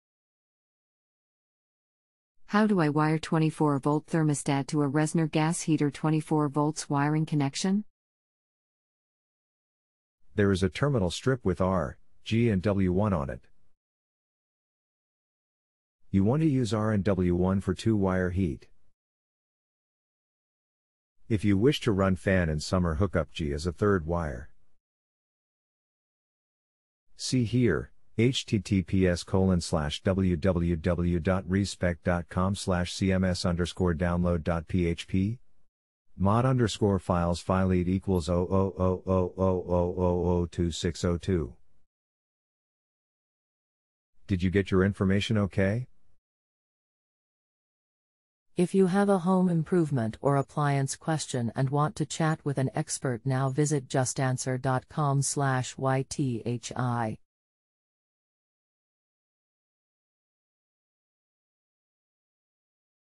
How to wire a 24-volt thermostat to a Reznor gas heater?
JustAnswer Customer: How do I wire 24-volt thermostat to a Reznor gas heater 24V wiring connection?
BillyHvac: Journeyman HVAC Tech: There is a terminal strip with R, G and W1 on it.
BillyHvac: Journeyman HVAC Tech: You want to use R and W1 for 2-wire heat.
BillyHvac: Journeyman HVAC Tech: If you wish to run fan in summer hook up G as a third wire.
BillyHvac: Journeyman HVAC Tech: Did you get your information OK?